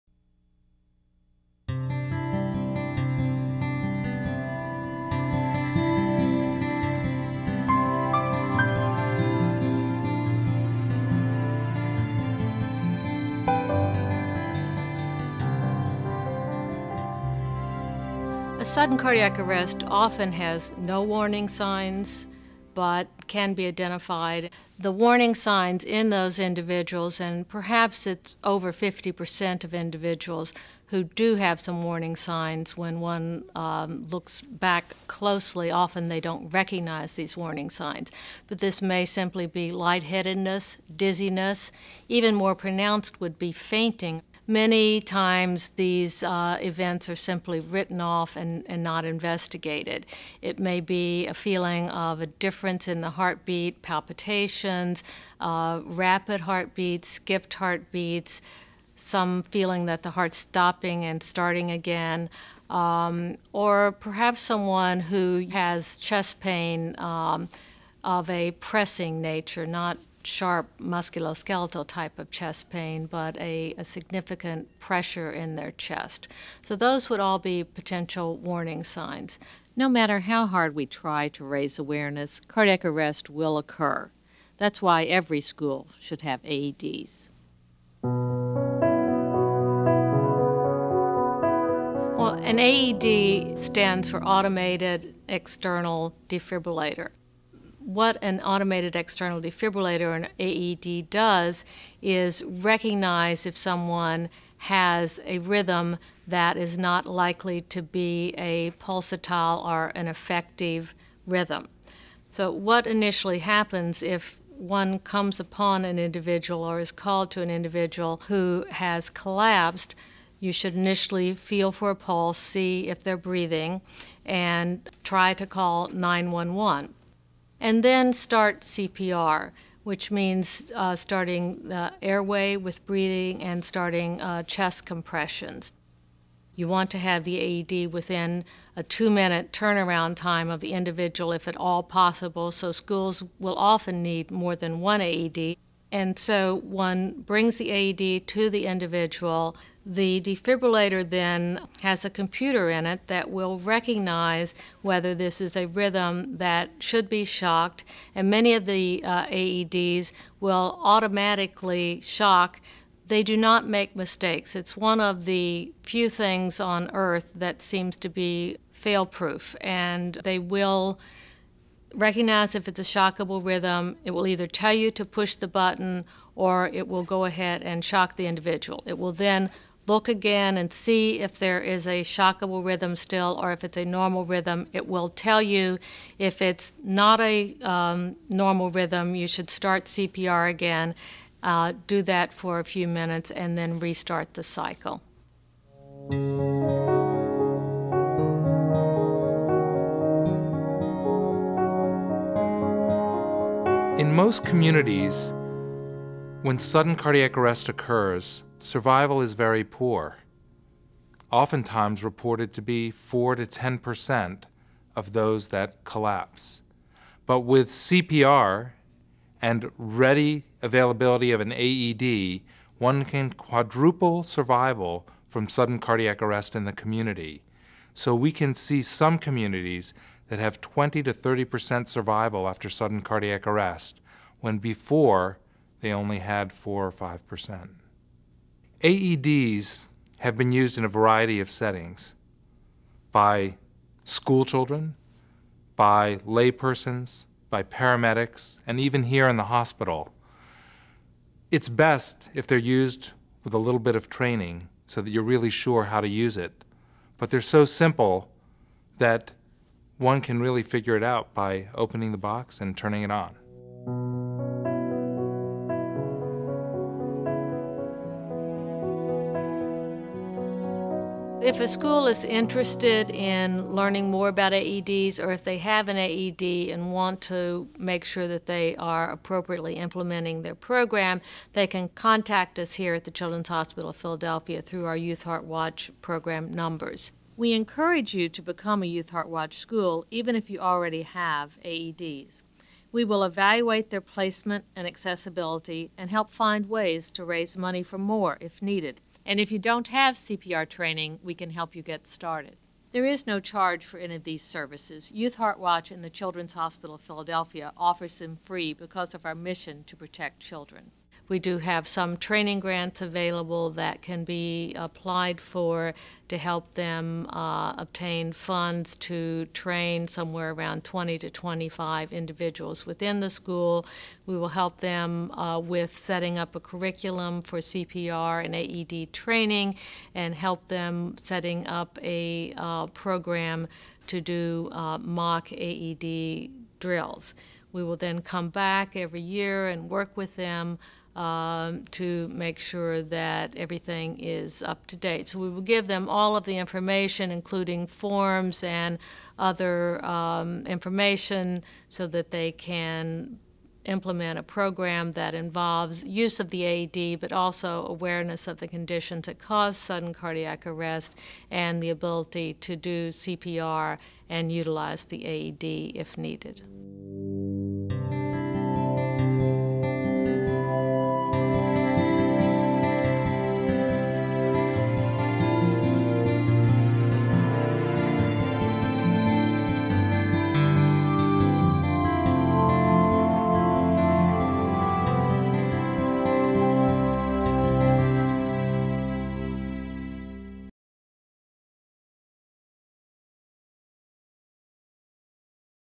Every Second Counts - Youth Heart Watch - The Children's Hospital of Philadelphia (2 of 3)
Information about AEDs and the risk factors and symptoms of sudden cardiac arrest.

This video series about sudden cardiac arrest shares the stories of several families whose children died unexpectedly. Learn more about sudden cardiac arrest and Youth Heart Watch, a program at The Children's Hospital of Philadelphia whose goal is to significantly reduce sudden cardiac arrest and death among children.